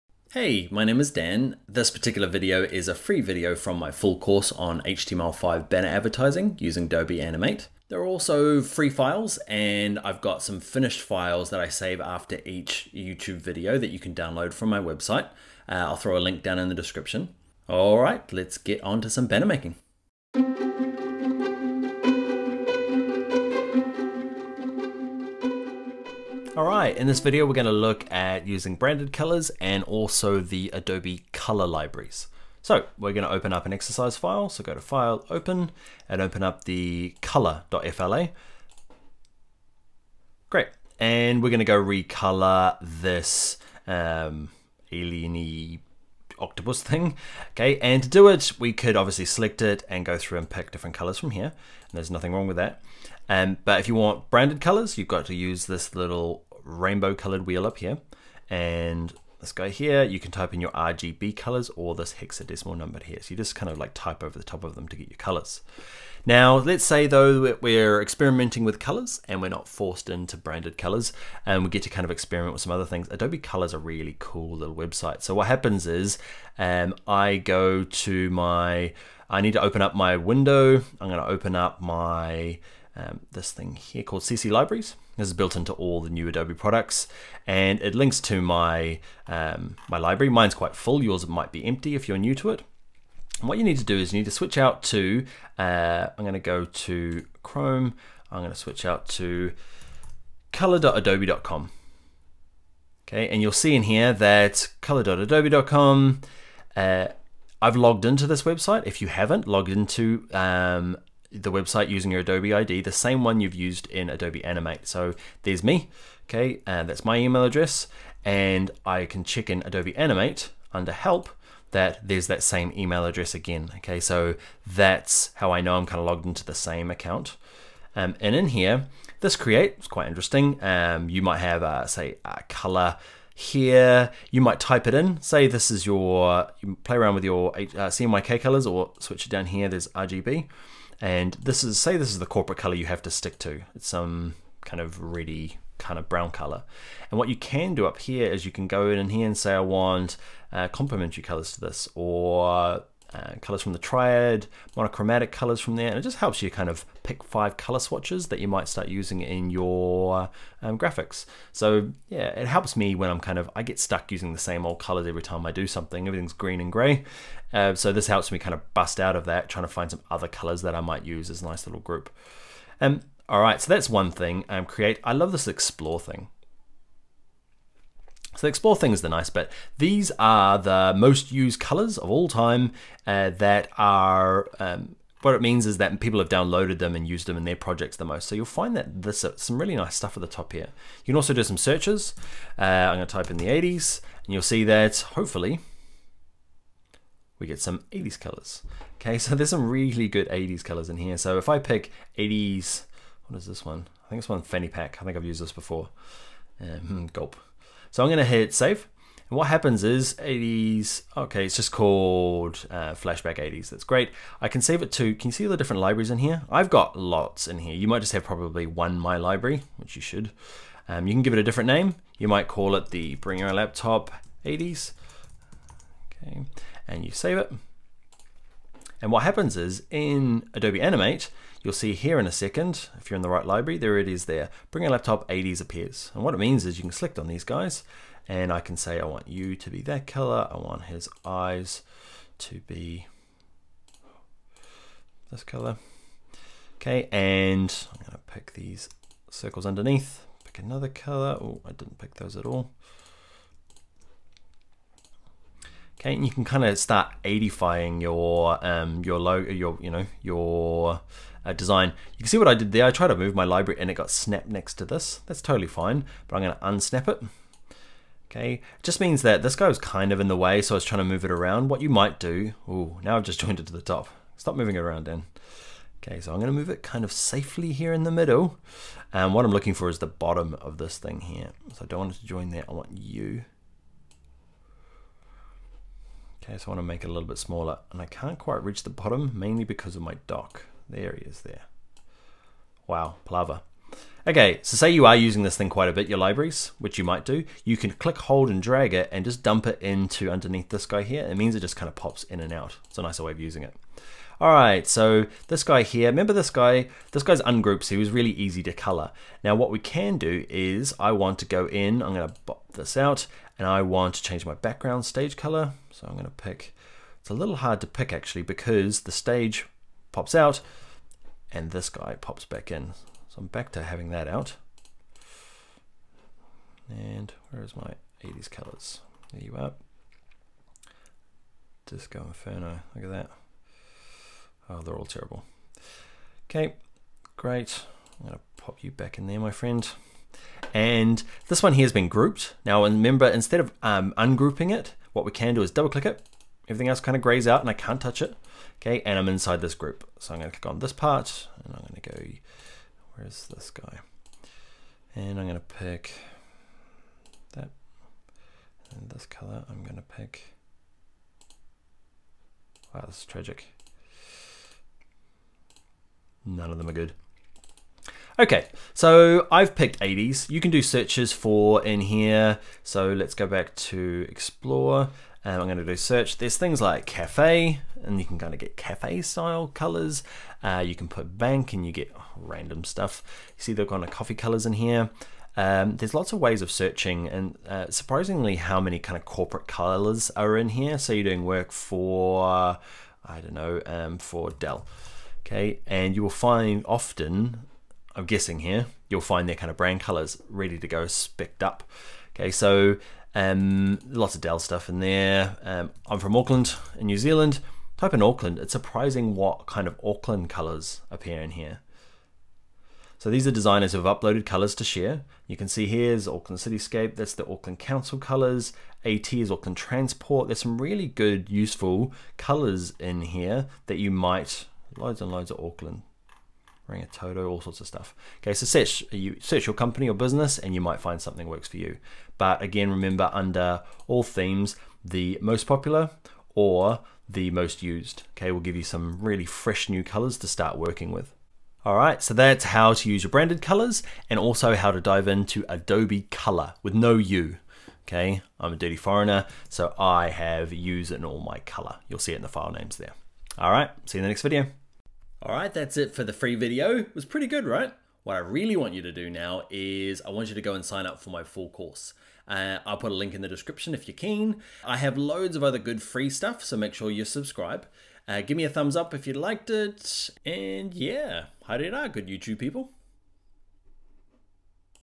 Using Brand Colours and Adobe Color - HTML5 Banner Ads in Adobe Animate [13/53]
In this episode we'll look at using Brand Colours from Adobe Color in our HTML5 Banner ads.

Created by Daniel Scott of Bring Your Own Laptop